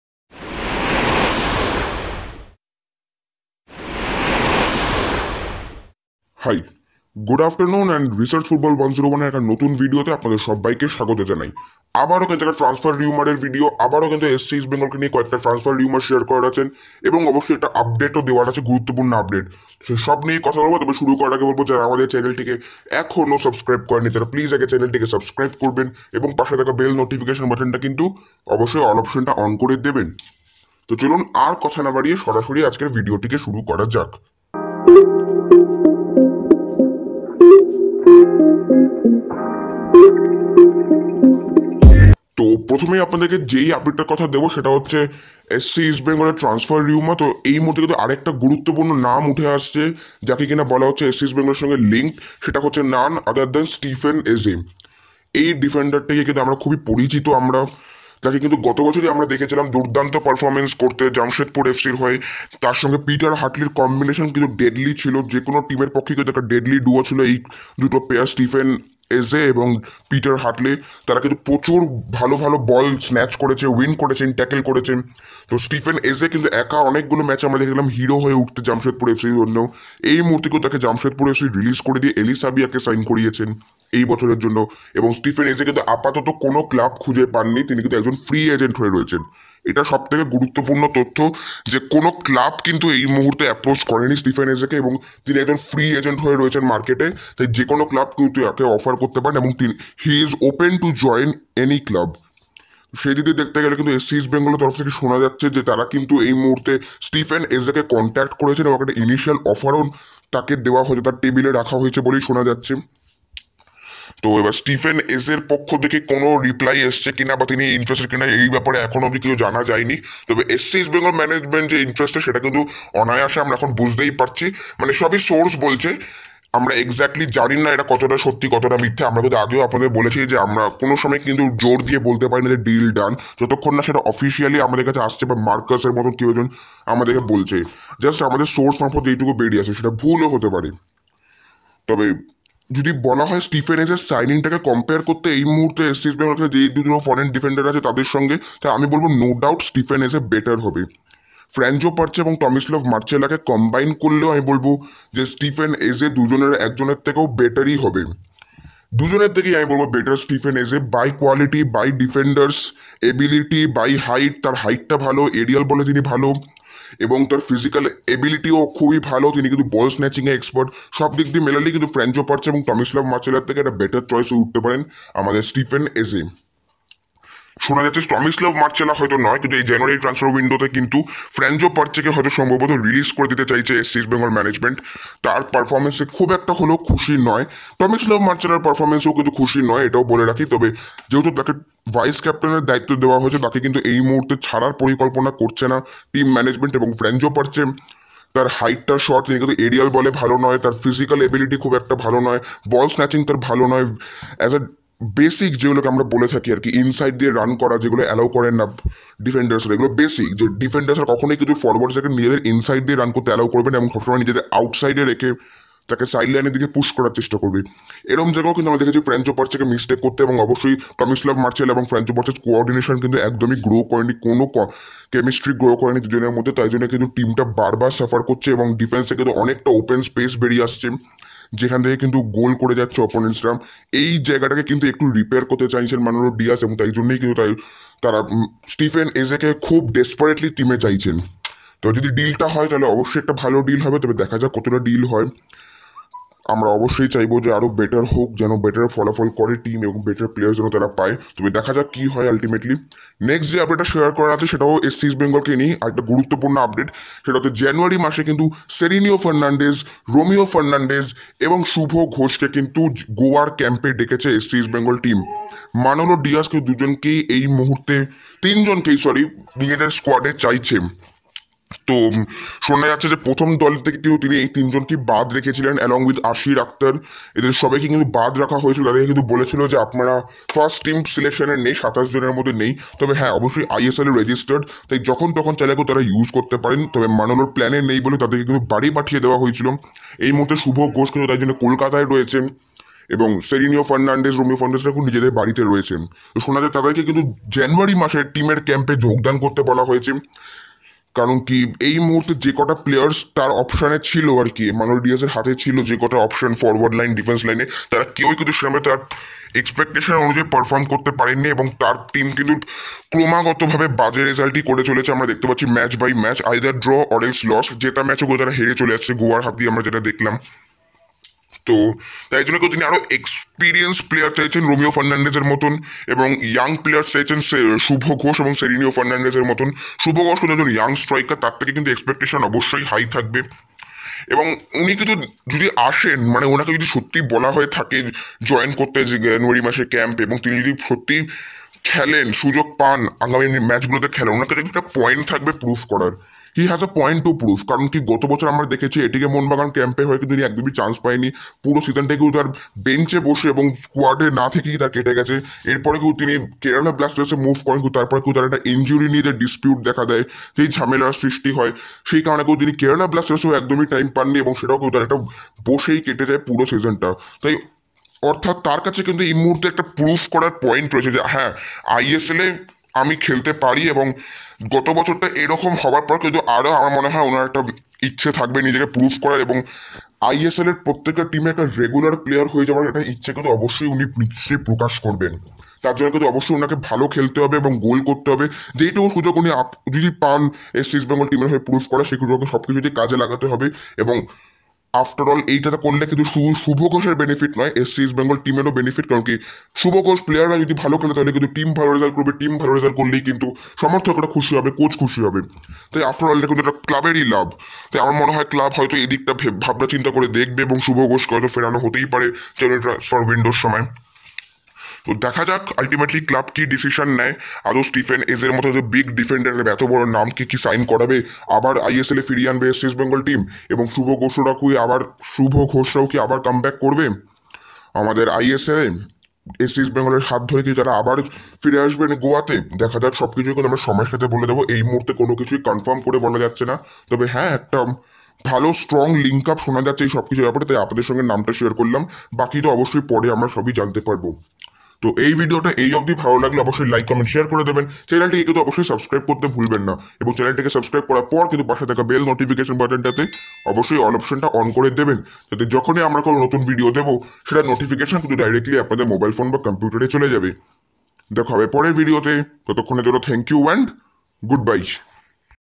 Stephen Eze to East Bengal? Transfer Rumour II Subho Ghosh returning in January !😍 East Bengal news
In this channel we discuss every aspects of football. We  discuss both Indian and world football .
In this video we discussed about ISL's transfer rumour.
Stephen Eze is likely to join SCEB this January. The former Jamshedpur FC defender is on the radar of SC East Bengal management. It is believed that the Nigerian centre back has been provided a contract offer from the SCEB team.

Subho Ghosh along with Romeo Fernandez and Serineo Fernandez may return to Goa camp in January. Manolo Diaz is now short of options and is thus looking for more players. These players were initially rejected from the first team list and thus they returned home. Now it is believed that they have been asked to join the camp in January.
Let's wait and see what happens.
If you wish you can join 🙏